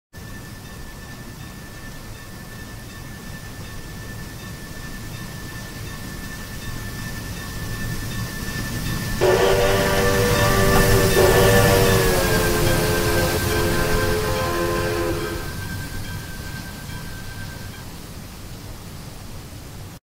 PRR K4s on the North Jersey Coast Line
Pennsylvania Railroad (PRR) K4s no. 3753 leads an excursion on the NJ Transit North Jersey Coastline from Bay Head to Hoboken right off the Northeast Corridor.